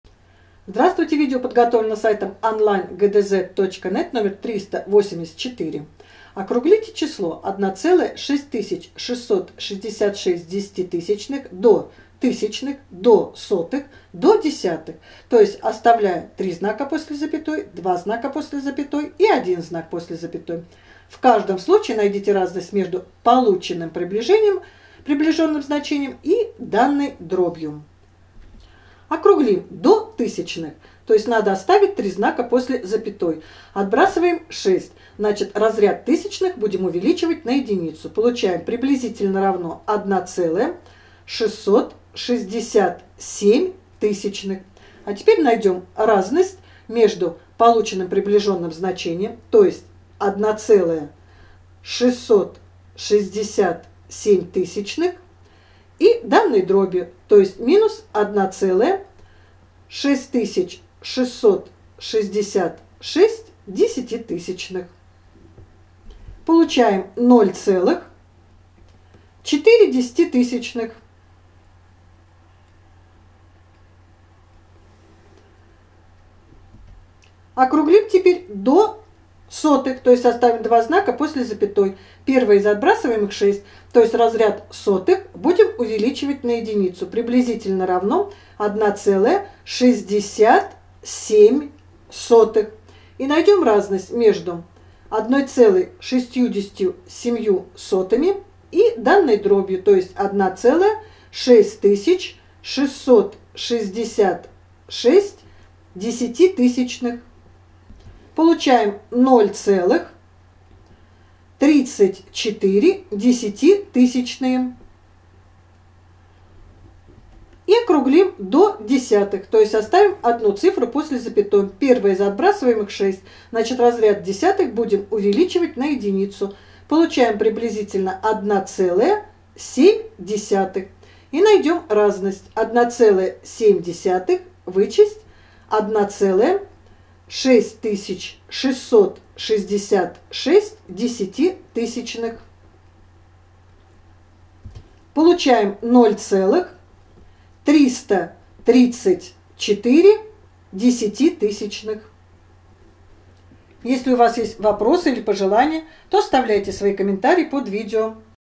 Задание №384 - ГДЗ по математике 6 класс (Дорофеев Г.В., Шарыгин И.Ф.)
Номер 384 – необходимо округлить данную десятичную дробь до указанных разрядов, применяя правило округления чисел, и найти разность между полученным приближенным результатом и данной дробью, применяя правило вычитания десятичных дробей.